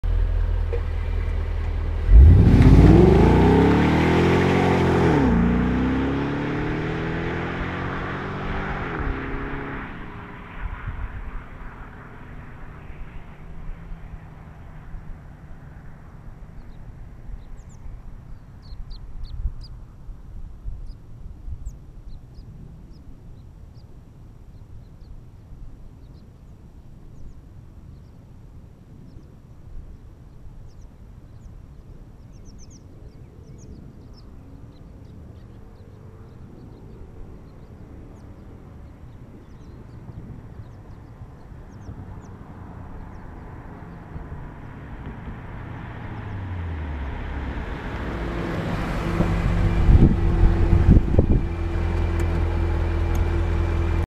2014 Mustang GT Stock Exhaust Sound (Part 1 of 6 NXTSTEP Exhaust Install)
(PART 1 OF 6 ) NXTSTEP Exhaust Install
The stock exhaust sound of a 2014 Mustang GT from outside. Filmed with a GoPro Hero3 with no case on.
About to install a NXTSTEP Exhaust